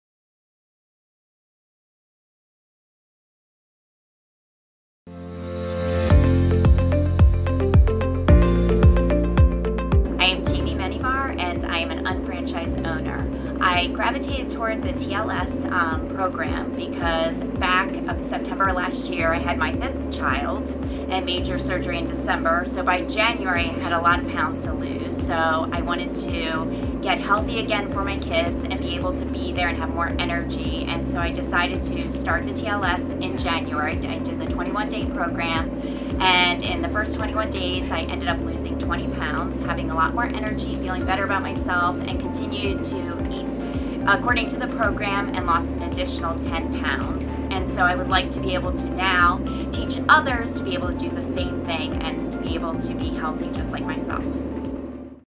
TLS Top 21 Coaches: Jaime Menjivar Interview
Hear advice and the compelling stories of the 2017 TLS Top 21 Coaches.